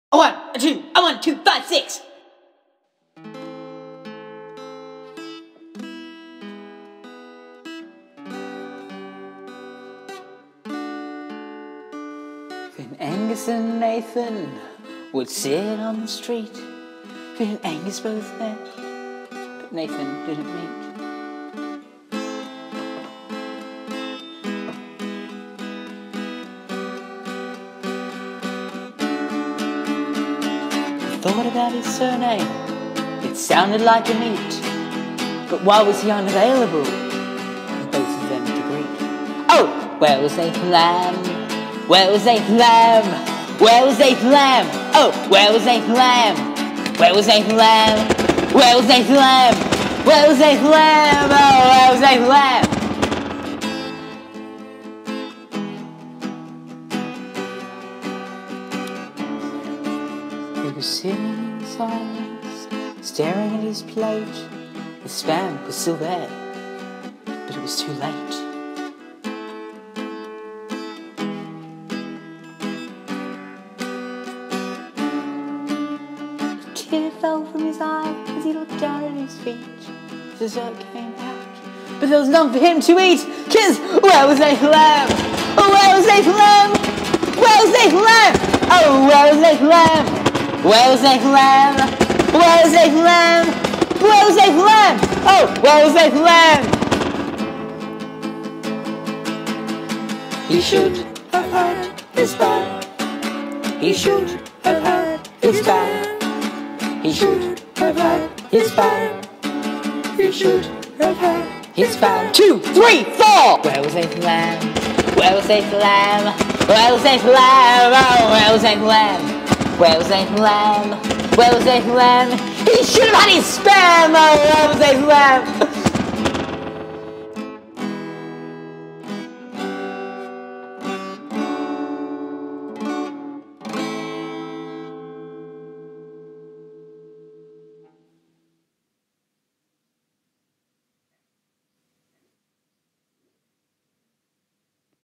Where was Nathan Lamb?
We have a friend named Nathan Lamb. We went to his house one day, and, he wasn't there. So we wrote a song about where he could be.